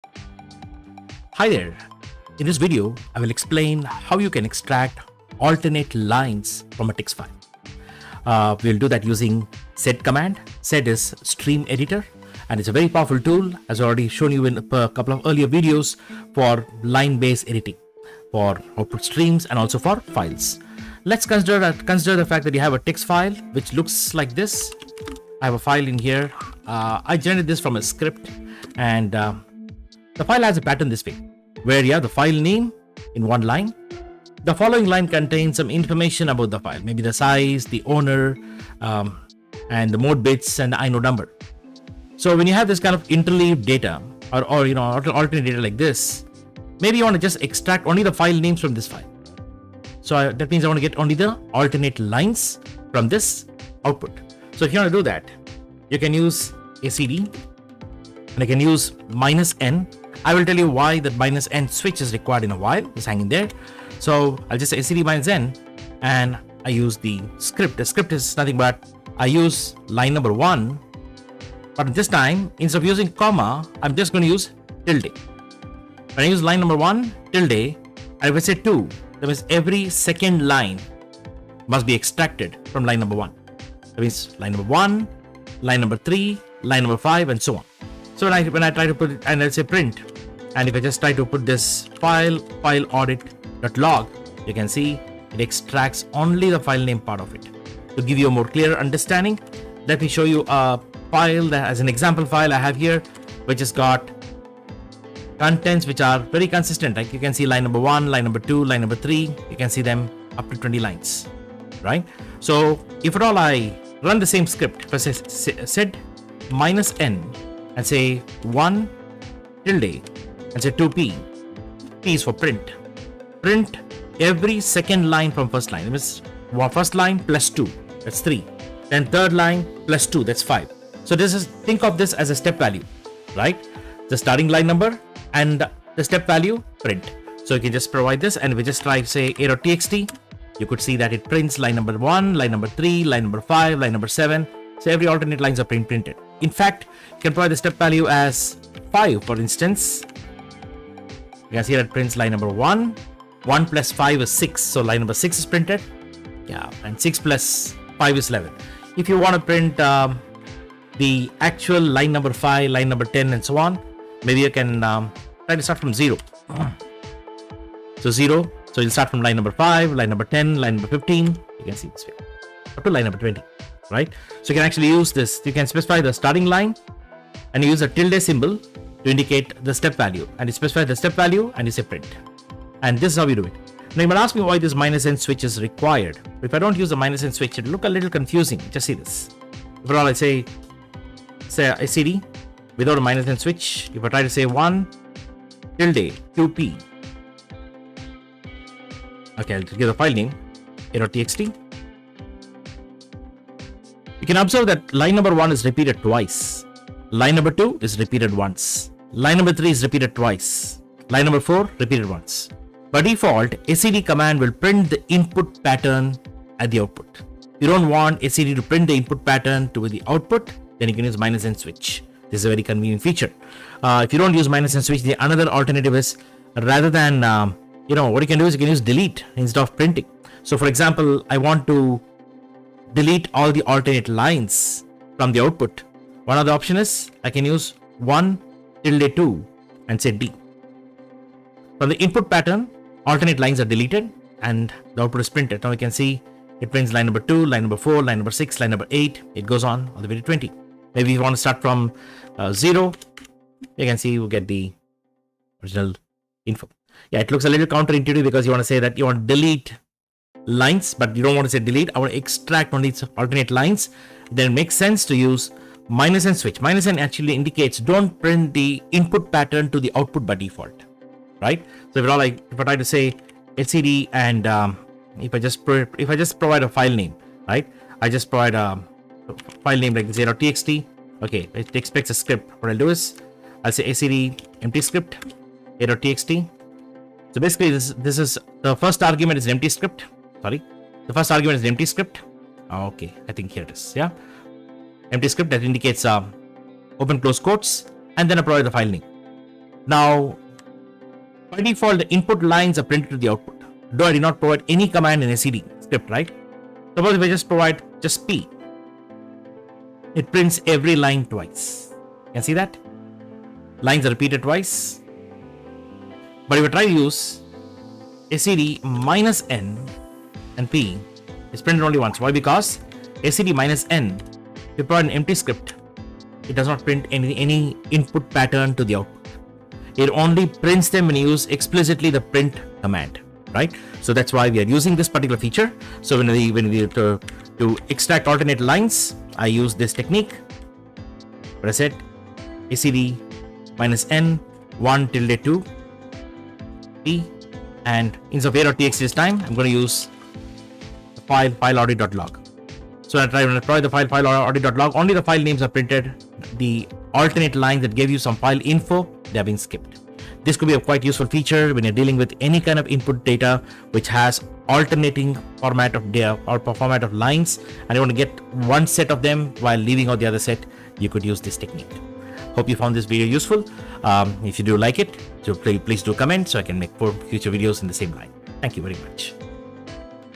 Linux command-line: extract alternate lines from a text file or command's output.
The 'sed' command is a versatile tool to edit output streams (command's output or text file contents) on Linux/UNIX environments.

In this video, you will learn how to use 'sed' effectively to extract alternate lines from a file. The same technique can be applied to the command's output when used as a filter.